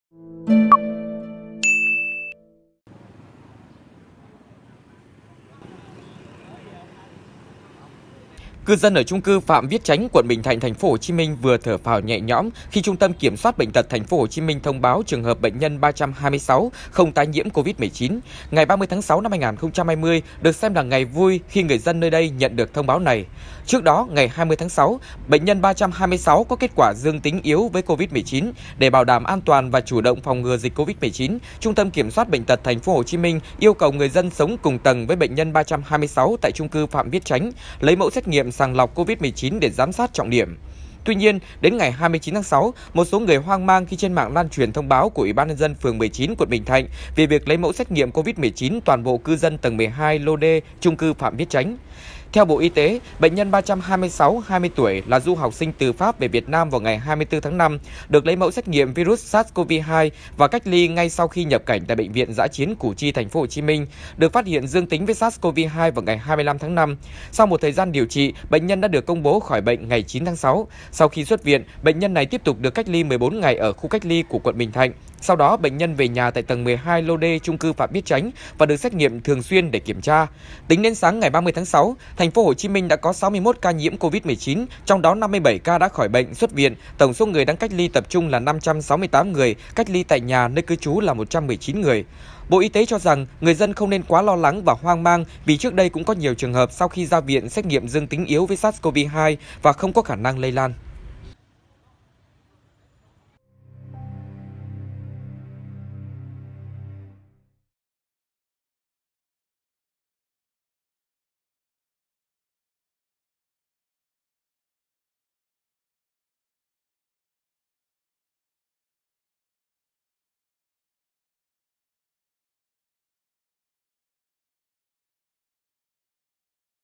Dân chung cư Phạm Viết Chánh đã ăn ngon ngủ yên sau nghi vấn bệnh nhân 326 tái nhiễm Covid-19
Cư dân ở chung cư Phạm Viết Chánh (quận Bình Thạnh, TP.HCM) vừa được thở phào nhẹ nhõm khi Trung tâm Kiểm soát bệnh tật TP.HCM thông báo trường hợp bệnh nhân 326 không tái nhiễm Covid-19.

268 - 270 Nguyễn Đình Chiểu, Phường 6, Q.3, TP HCM